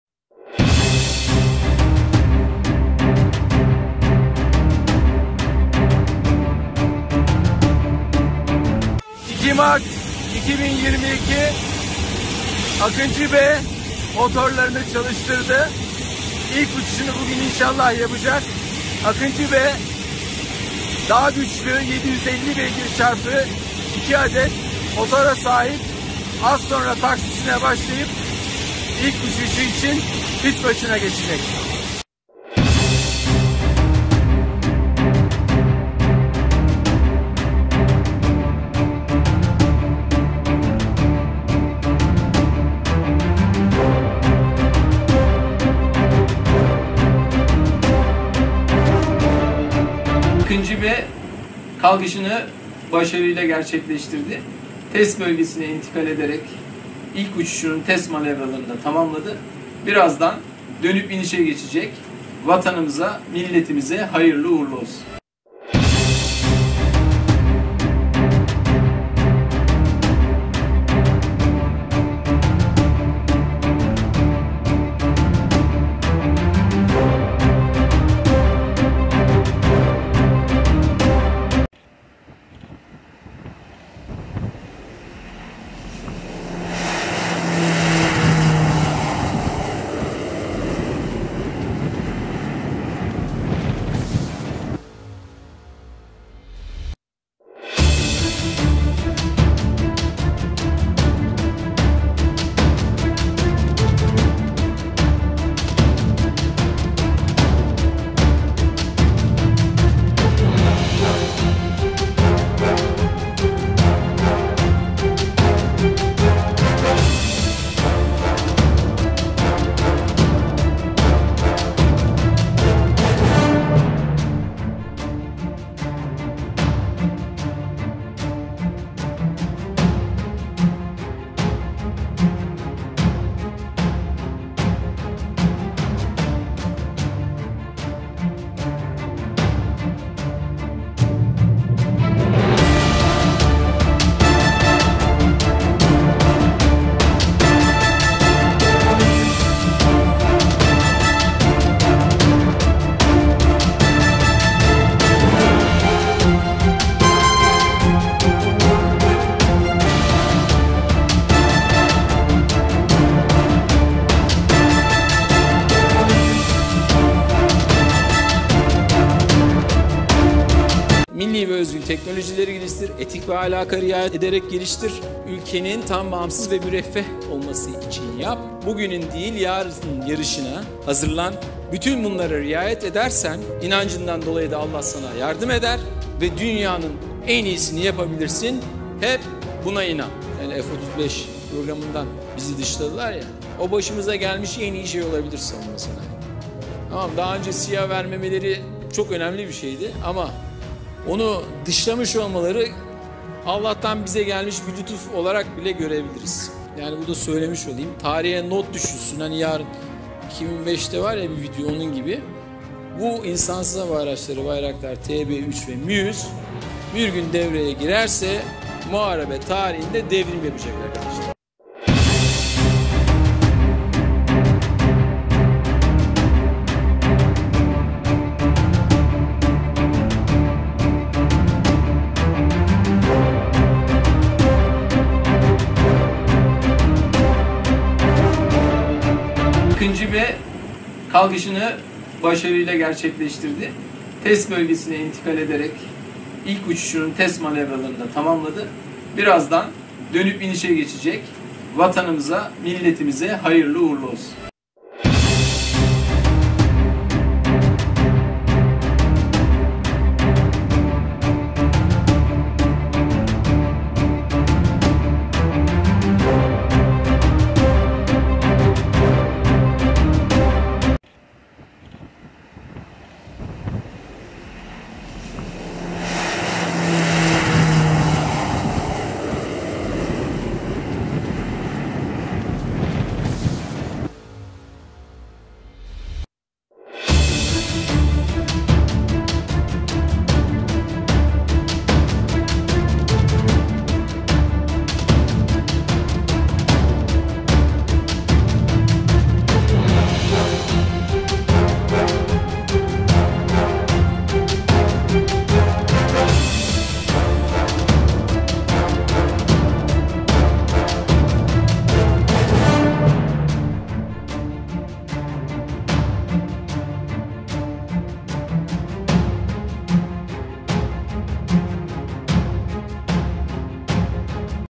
AKINCI B Kendisine Ait Olan İrtifa Rekorunu Az Önce Kırdı. Bayraktar AKINCI-B TİHA! Mutlaka İzle
Selçuk Bayraktar, dünyanın hayran kaldığı AKINCI TİHA'yla ilgili gurur veren yeni gelişmeyi duyurdu.

Bayraktar, BAYKAR tarafından yerli ve milli olarak üretilen Türkiye'nin yeni gücü AKINCI TİHA B'nin kendisine ait olan rekoru kırdığını açıkladı.